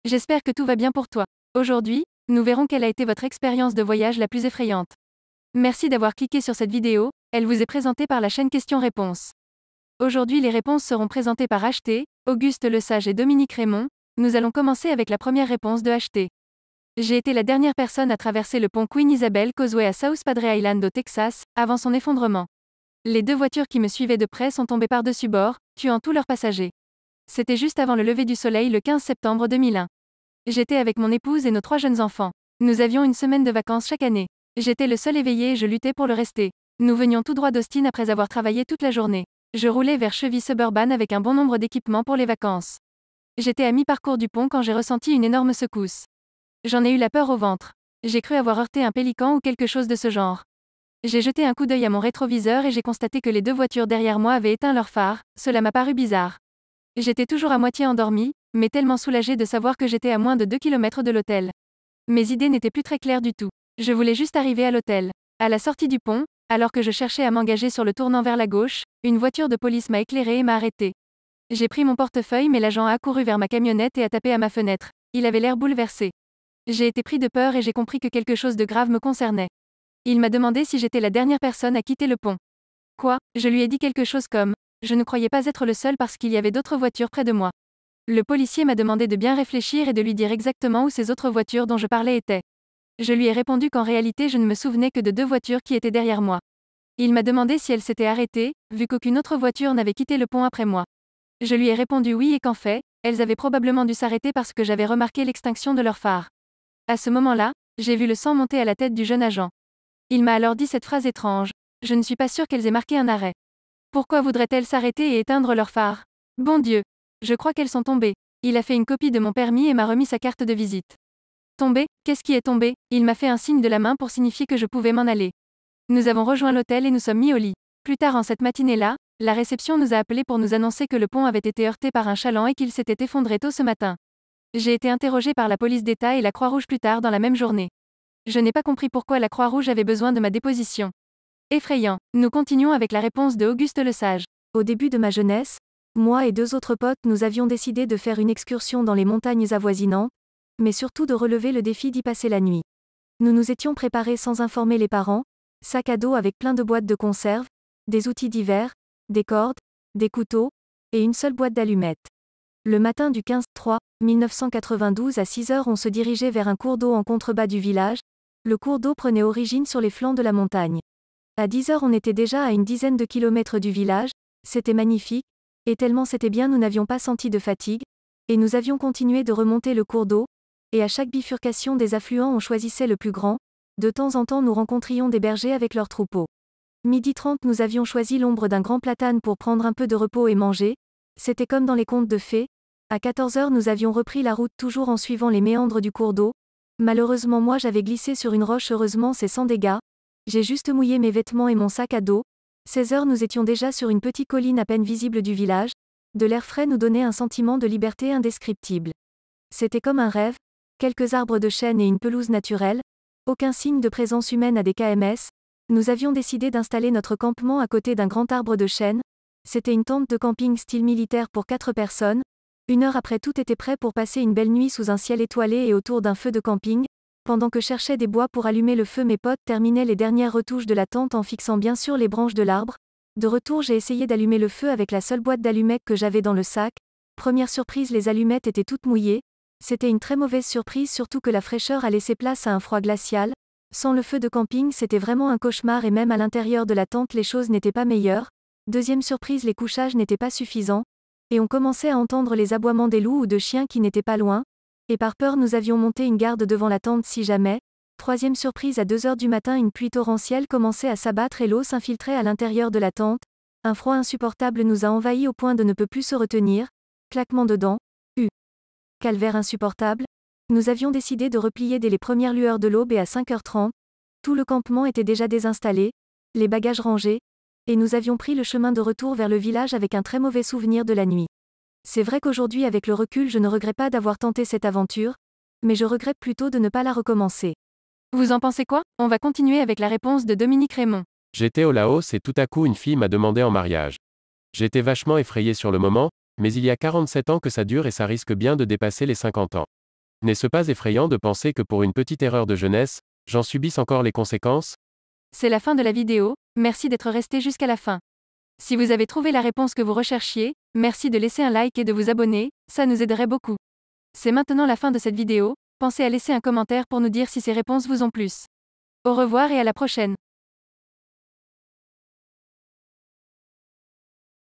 Quelle a été votre expérience de voyage la plus effrayante ?
- H T:
 J'ai été la dernière personne à traverser le pont Queen Isabelle Causeway à South Padre Island au Texas, avant son effondrement. Les deux voitures qui me suivaient de près sont tombées par dessus bord, tuant tous leurs passagers. C'était juste avant le lever du soleil le 15 septembre 2001. J'étais avec mon épouse et nos trois jeunes enfants. Nous avions une semaine de vacances chaque année. J'étais le seul éveillé et je luttais pour le rester. Nous venions tout droit d'Austin après avoir travaillé toute la journée. Je roulais vers Chevy Suburban avec un bon nombre d'équipements pour les vacances. J'étais à mi parcours du pont quand j'ai ressenti une énorme secousse. J'en ai eu la peur au ventre. J'ai cru avoir heurté un pélican ou quelque chose de ce genre. J'ai jeté un coup d'œil à mon rétroviseur et j'ai constaté que les deux voitures derrière moi avaient éteint leurs phares, cela m'a paru bizarre. J'étais toujours à moitié endormi, mais tellement soulagé de savoir que j'étais à moins de 2 km de l'hôtel. Mes idées n'étaient plus très claires du tout. Je voulais juste arriver à l'hôtel. À la sortie du pont, alors que je cherchais à m'engager sur le tournant vers la gauche, une voiture de police m'a éclairé et m'a arrêté. J'ai pris mon portefeuille mais l'agent a accouru vers ma camionnette et a tapé à ma fenêtre ; il avait l'air bouleversé. J'ai été pris de peur et j'ai compris que quelque chose de grave me concernait. Il m'a demandé si j'étais la dernière personne à quitter le pont. Quoi ? Je lui ai dit quelque chose comme : je ne croyais pas être le seul parce qu'il y avait d'autres voitures près de moi. Le policier m'a demandé de bien réfléchir et de lui dire EXACTEMENT où ces autres voitures dont je parlais étaient. Je lui ai répondu qu'en réalité je ne me souvenais que de deux voitures qui étaient derrière moi. Il m'a demandé si elles s'étaient arrêtées, vu qu'aucune autre voiture n'avait quitté le pont après moi. Je lui ai répondu oui et qu'en fait, elles avaient probablement dû s'arrêter parce que j'avais remarqué l'extinction de leurs phares. À ce moment-là, j'ai vu le sang monter à la tête du jeune agent. Il m'a alors dit cette phrase étrange : "Je ne suis pas sûr qu'elles aient marqué un arrêt. Pourquoi voudraient-elles s'arrêter et éteindre leurs phares ? Bon Dieu! Je crois qu'elles sont tombées". Il a fait une copie de mon permis et m'a remis sa carte de visite. Tombées ? Qu'est-ce qui est tombé ? Il m'a fait un signe de la main pour signifier que je pouvais m'en aller. Nous avons rejoint l'hôtel et nous sommes mis au lit. Plus tard en cette matinée-là, la réception nous a appelés pour nous annoncer que le pont avait été heurté par un chaland et qu'il s'était effondré tôt ce matin. J'ai été interrogé par la police d'État et la Croix-Rouge plus tard dans la même journée. Je n'ai pas compris pourquoi la Croix-Rouge avait besoin de ma déposition. Effrayant.

 - Auguste Lesage:
 Au début de ma jeunesse, moi et deux autres potes nous avions décidé de faire une excursion dans les montagnes avoisinantes, mais surtout de relever le défi d'y passer la nuit. Nous nous étions préparé sans informé les parents, sacs à dos avec plein de boîtes de conserve , des outils divers , des cordes , des couteaux, et une seule boîte d'allumettes. Le matin du 15/3/1992 à 6 h on se dirigeait vers un cours d'eau en contrebas du village , le cours d'eau prenait origine sur les flancs de la montagne. À 10 h on était déjà a une dizaine de km du village , c'était magnifique , et tellement c'était bien nous n'avions pas senti de fatigue, et nous avions continué de remonter le cours d'eau , et à chaque bifurcation des affluents on choisissait le plus grand , de temps en temps nous rencontrions des bergers avec leurs troupeaux. 12h30 nous avions choisi l'ombre d'un grand platane pour prendre un peu de repos et manger, c'était comme dans les contes de fées , à 14 h nous avions repris la route toujours en suivant les méandres du cours d'eau, malheureusement moi j'avais glissé sur une roche heureusement c'est sans dégâts , j'ai juste mouillé mes vêtements et mon sac à dos , 16 h nous étions déjà sur une petite colline à peine visible du village , de l'air frais nous donnait un sentiment de liberté indescriptible. C'était comme un rêve, quelques arbres de chêne et une pelouse naturelle , aucun signe de présence humaine à des kms , nous avions décidé d'installer notre campement à côté d'un grand arbre de chêne, c'était une tente de camping style militaire pour 4 personnes, une heure après tout était prêt pour passez une belle nuit sous un ciel étoilé et autour d'un feu de camping , pendant que cherchais des bois pour allumer le feu mes potes terminaient les dernières retouches de la tante en fixant bien sûr les branches de l'arbre , de retour j'ai essayé d'allumer l

Vidéo éditée automatiquement par QRBot.